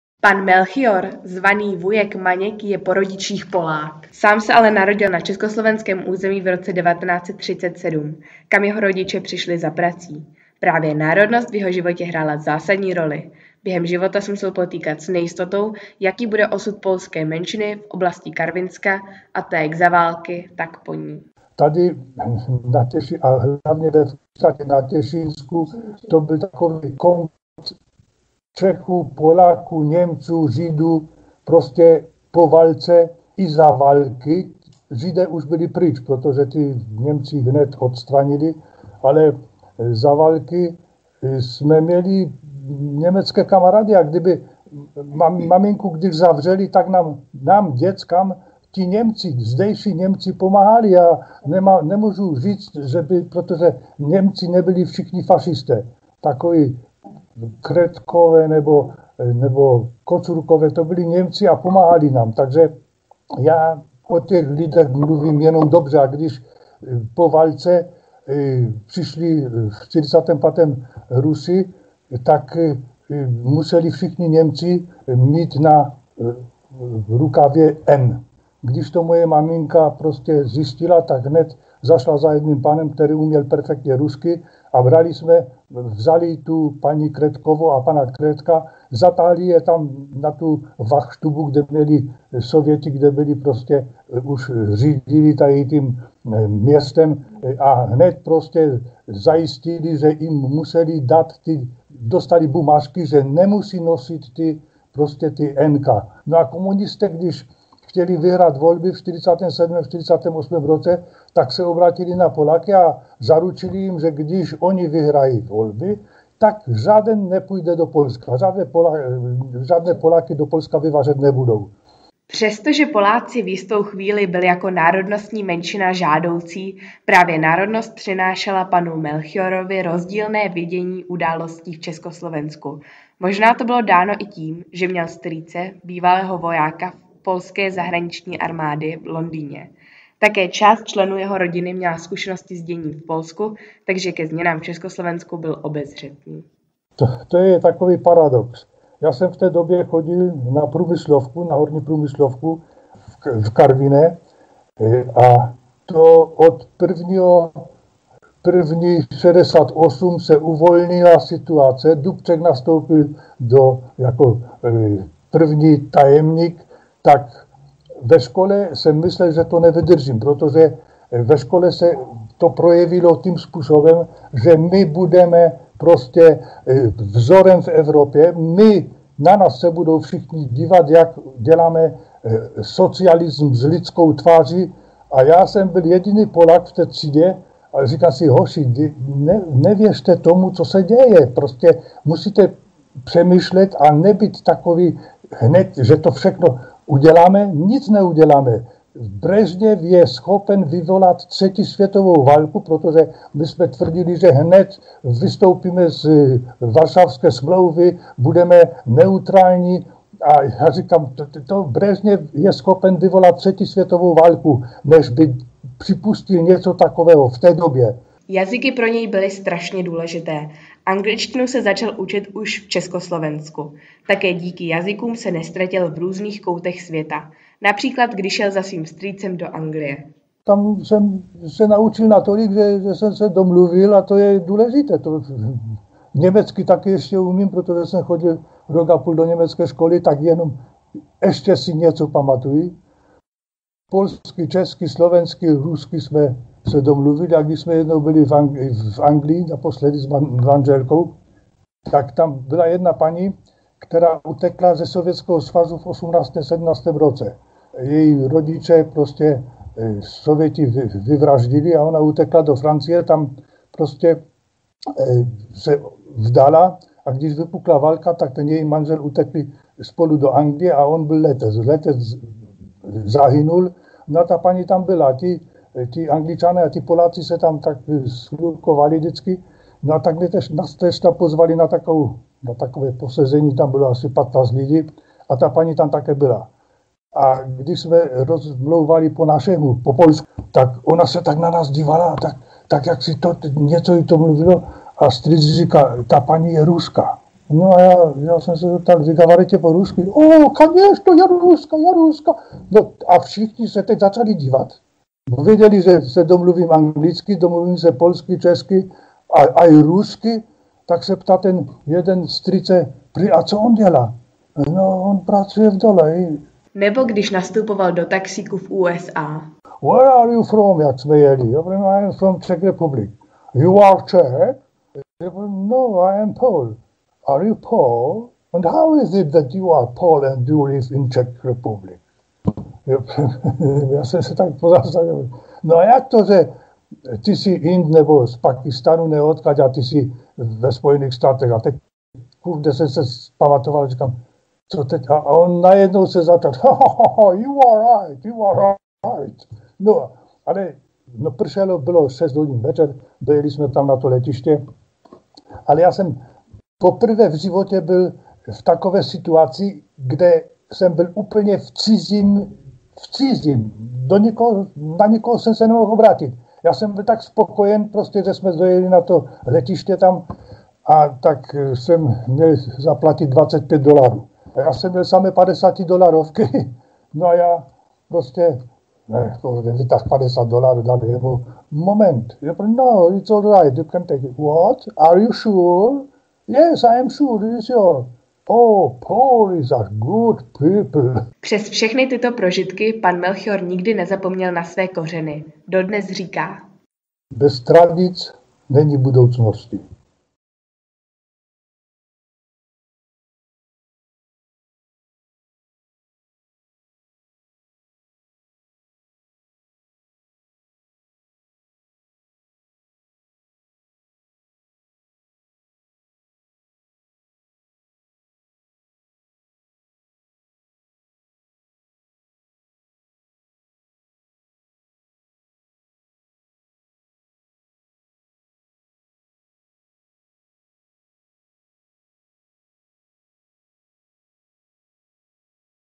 Melchior Sikora: Pamětník polské menšiny v Československu
V rámci projektu Příběhy našich sousedů organizace Post Bellum natočili žáci ze ZŠ Brno, Kuldova